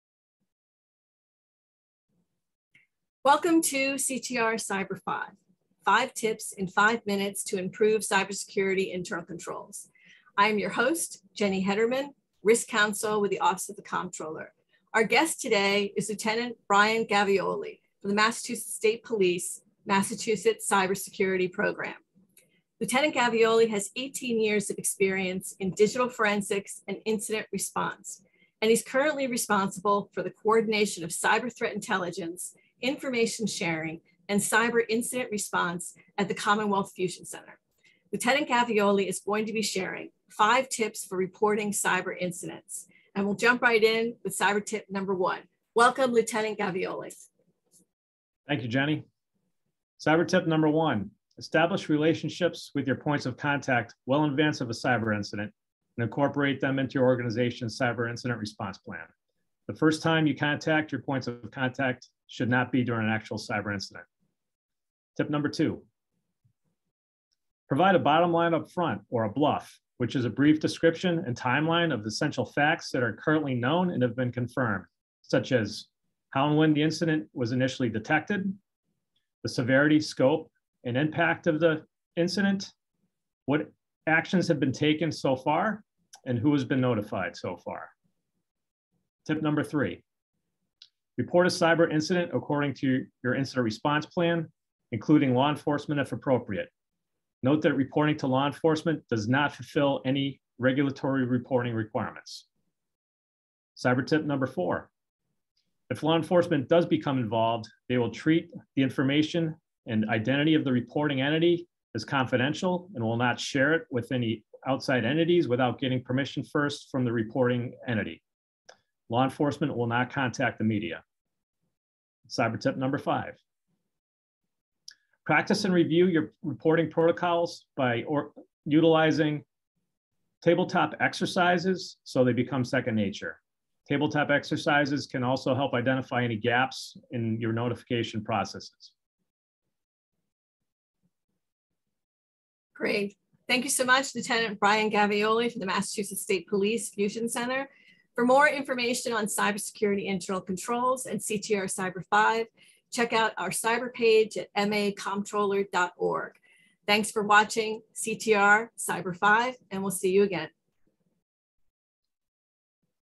The CTR Cyber 5: Lt. Brian Gavioli on Reporting Incidents
Lieutenant Brian Gavioli of the Massachusetts State Police returns to the CTR Cyber 5 to discuss tips for reporting incidents.

Lieutenant Gavioli has 18 years of experience in digital forensics and incident response, and responsible for the coordination of cyber threat intelligence, information sharing and cyber incident response at the Commonwealth Fusion Center.